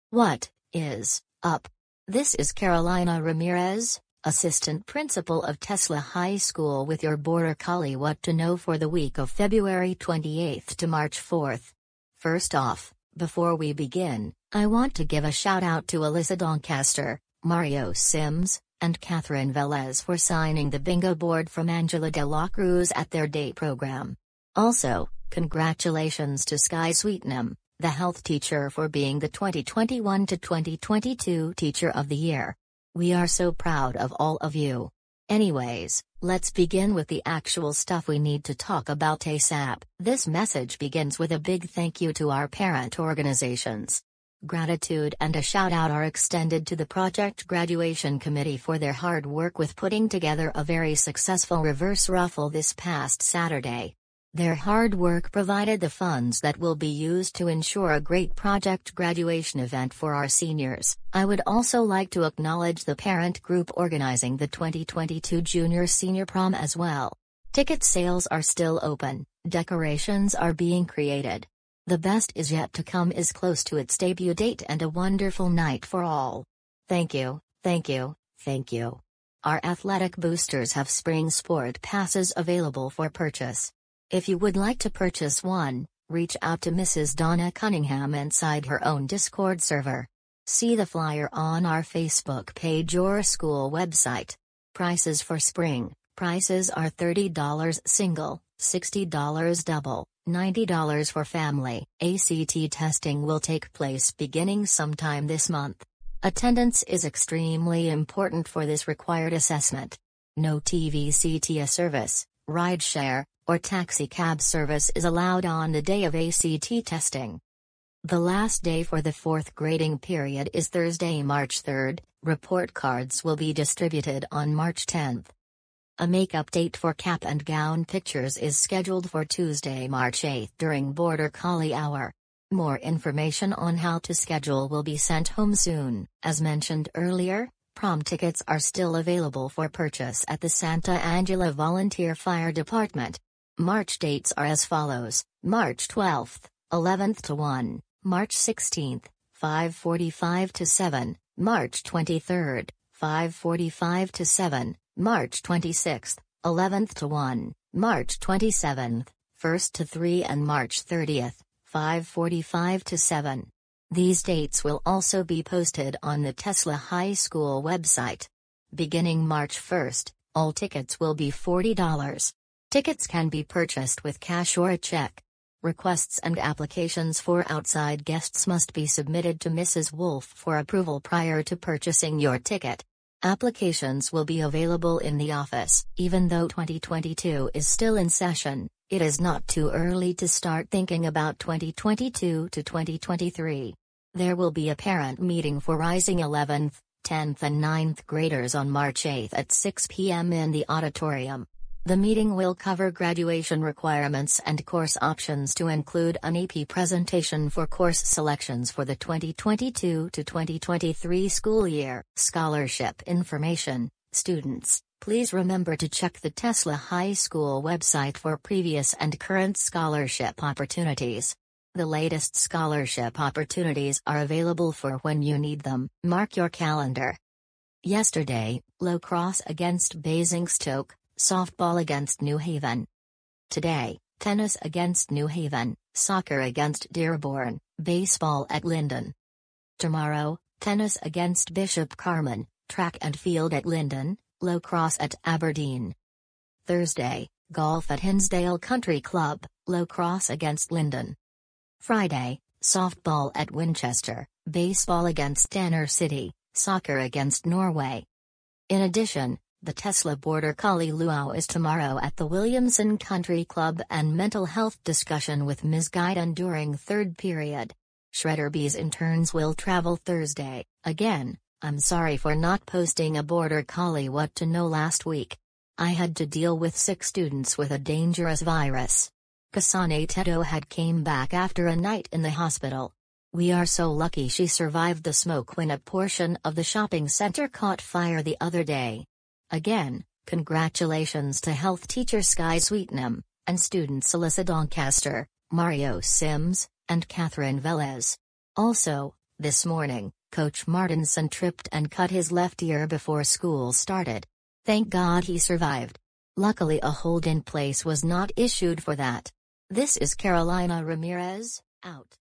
Mrs. Ramirez's Border Collie What-To-Know for 3/1/2022 (not 2/22/2022)
Mrs. Ramirez discusses what's going on in the Tesla High community for this week. Also, a message of Tailsland Martinson, a PE teacher there. Luckily, 5 students (all of which are the Enchantimals) helped him up this morning before school started.